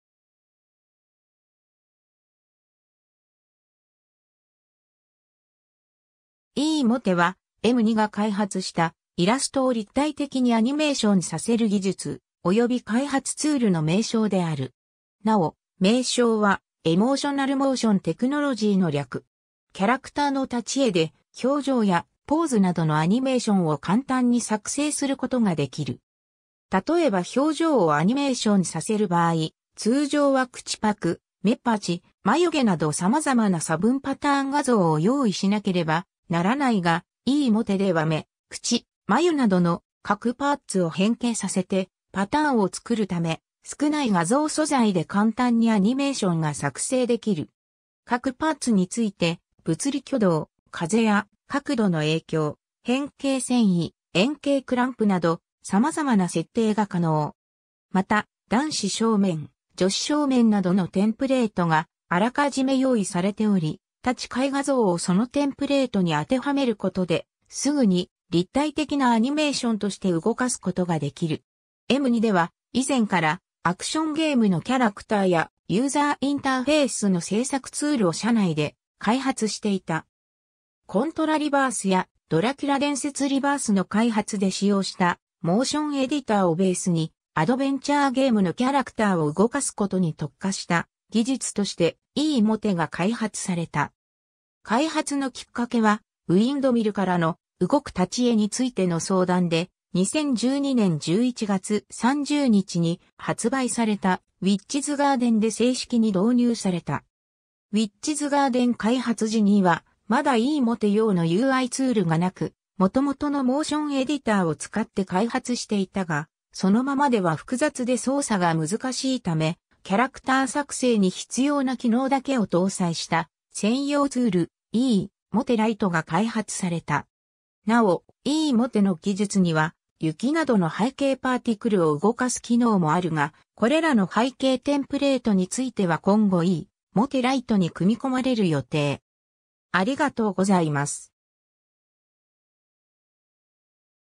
E-mote
漫画・アニメの表現
E-mote（エモート）は、M2が開発した、イラストを立体的にアニメーションさせる技術、および開発ツールの名称である。
なお、名称は「Emotional Motion Technology」の略。
 キャラクターの立ち絵で、表情やポーズなどのアニメーションを簡単に作成することができる。
例えば表情をアニメーションさせる場合、通常は口パク・目パチ・眉毛など様々な差分パターン画像を用意しなければならないが、E-moteでは目・口・眉などの各パーツを変形させてパターンを作るため、少ない画像素材で簡単にアニメーションが作成できる。
各パーツについて、物理挙動（硬さ・重さ・揺れ具合）、風や角度の影響、変形遷移・円形クランプなど、様々な設定が可能。
また「男子正面」「女子正面」などのテンプレートがあらかじめ用意されており、立ち絵画像をそのテンプレートに当てはめることで、すぐに立体的なアニメーションとして動かすことができる。
 M2では以前から、アクションゲームのキャラクターやユーザーインターフェイスの制作ツールを社内で開発していた。
『魂斗羅ReBirth』や『ドラキュラ伝説 ReBirth』の開発で使用したモーションエディターをベースに、アドベンチャーゲームのキャラクターを動かすことに特化した技術として「E-mote」が開発された。
 開発のきっかけはういんどみるからの「動く立ち絵」についての相談で、2012年11月30日に発売された『ウィッチズガーデン』（ういんどみるOasis）で正式に導入された。
『ウィッチズガーデン』開発時にはまだE-mote用のUIツールが無く、元々のモーションエディターを使って開発していたが、そのままでは複雑で操作が難しいため、キャラクター作成に必要な機能だけを搭載した専用ツール「E-mote Lite」が開発された。
 なお、E-moteの技術には、雪などの背景パーティクルを動かす機能もあるが、これらの背景テンプレートについては今後E-mote Liteに組み込まれる予定。
 （出典：）